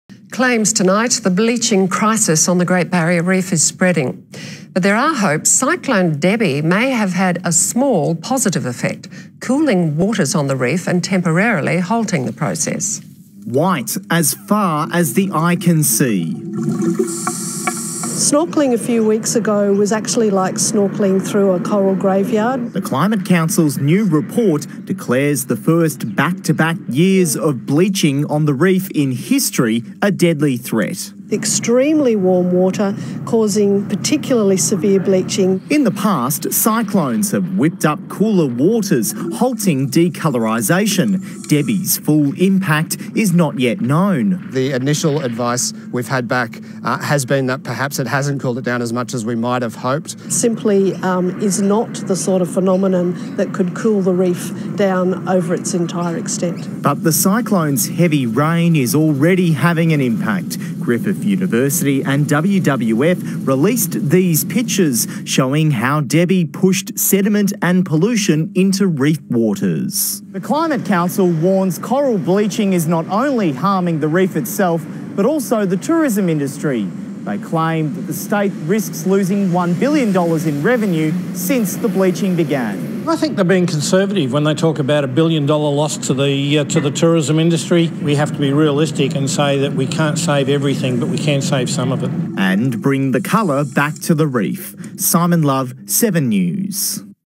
TC Debbie may have cooled waters over Great Barrier Reef but full impact not known
12/04/2017 Sediment and pollution flowing into Great Barrier Reef from TC Debbie.